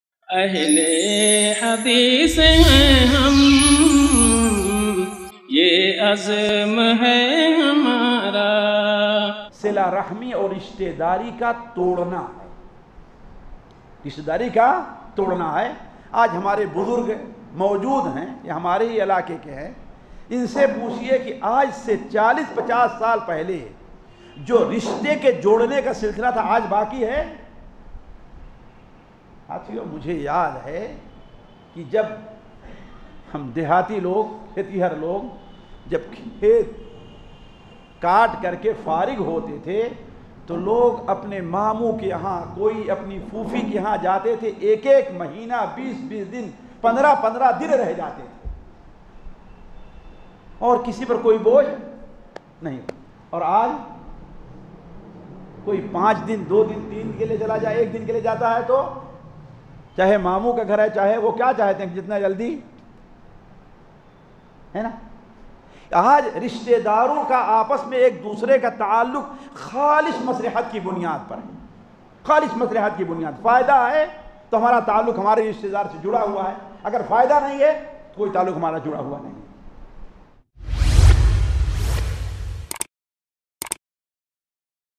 Rishtedari Ka Todna Ye Qayamat Ki Ek Nishanii Hai By Shaikh Maqsood ul Hasan Faizi
Jamiyyat Ahle Hadees,  Aurangabad

Presents

| Deeni Ijtema |

On The Topic of : Alamaat e Qayamat

Honorable Speaker:  Shaikh Shaikh Maqsood ul Hasan Faizi

Delivered On : Sunday 10 November 2019

Venue : Muhammadi Masjid, Champa Chowk, Roshan Gate Road, Aurangabad, Mh.